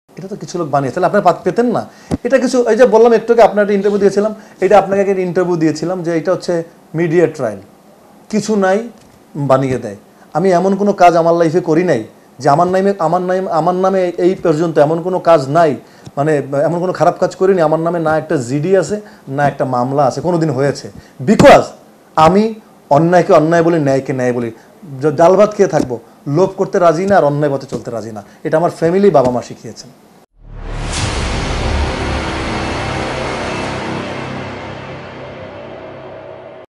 আমার নামে কোন জিডি বা মামলা নেই ।আমি কখনো কোন খারাপ কাজ করিনি । জায়েদ খান
Presenting আমার নামে কোন জিডি বা মামলা নেই ।আমি কখনো কোন খারাপ কাজ করিনি । জায়েদ খান।exclusive new interview  on our channel Creative buzz. Hope you will enjoy our upcoming movie shooting scenes.

*WARNING ANTI PIRACY*
Category